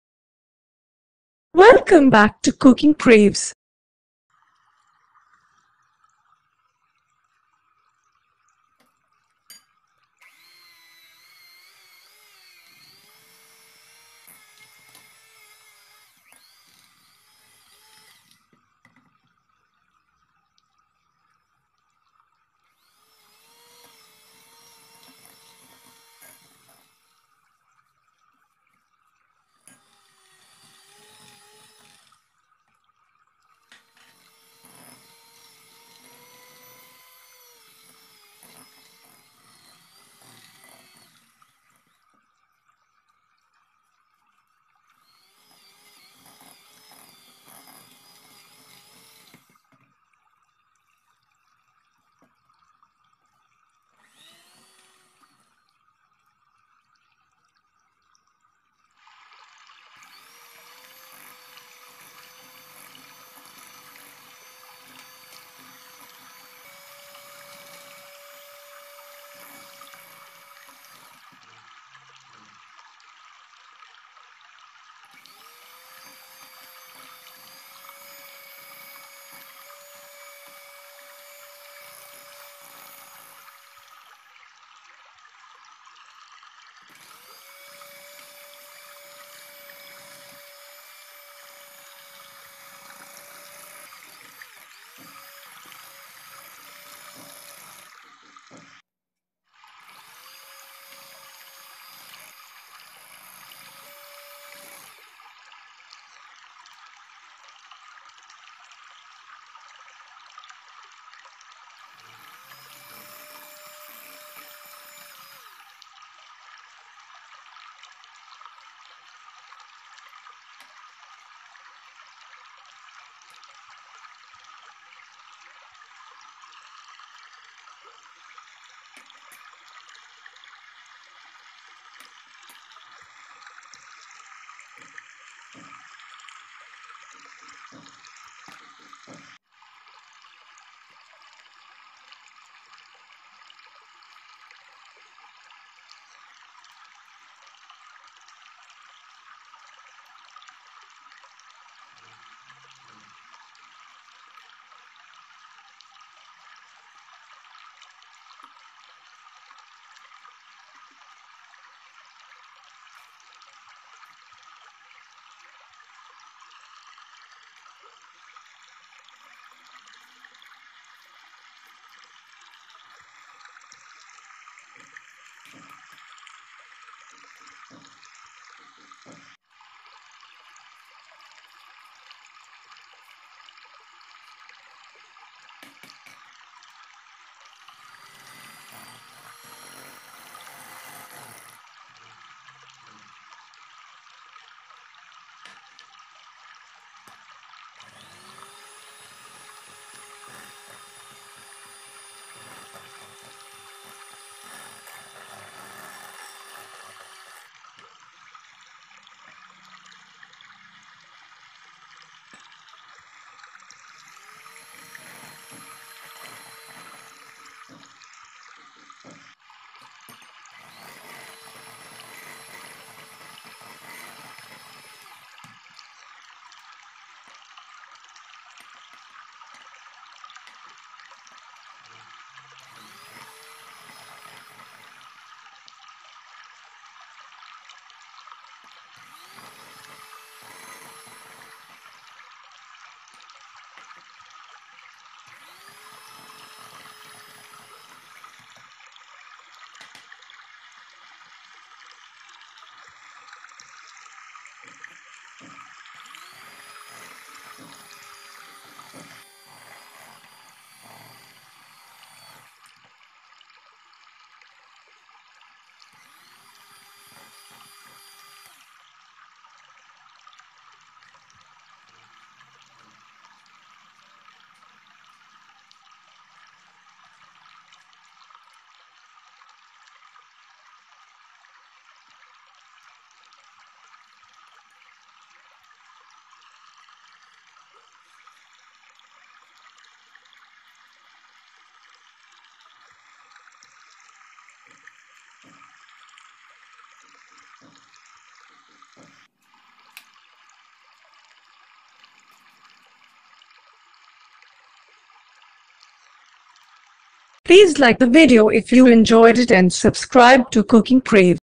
Decorate your eid table with Sri Lankan Style Ribbon Cake /රිබන් කේක් Ribbon cake with cream recipe
Ribbon cakes are not only delicious,but also  makes the whole table looks aesthetic with its pretty layers.
The complete list of ingredients is given at the end of the video.
Try this and please post your comments.